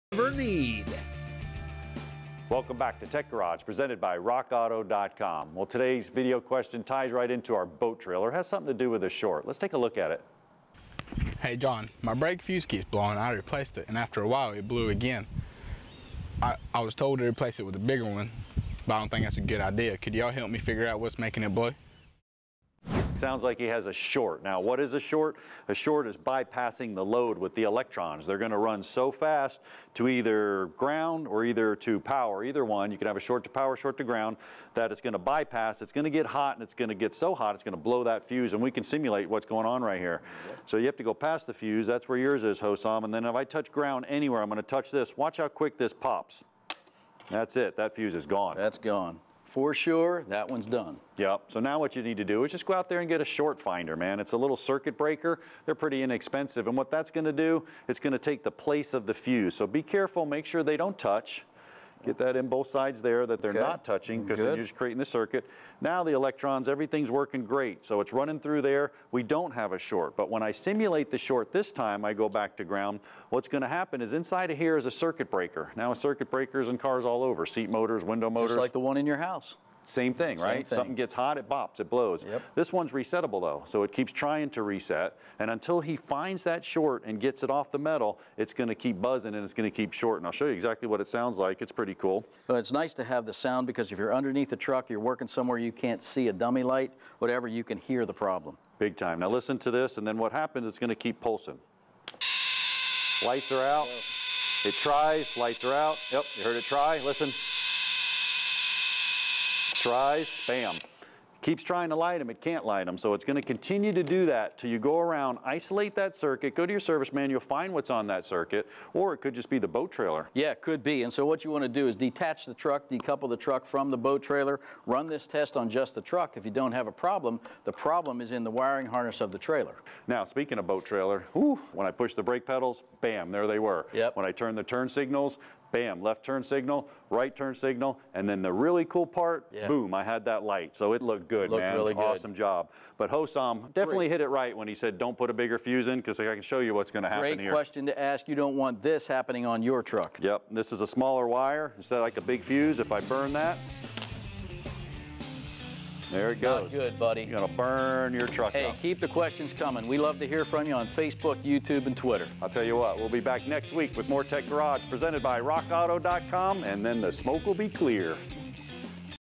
Wiring Short How to Diagnose & Locate
Bryan & John teach a student how to track down his wiring problem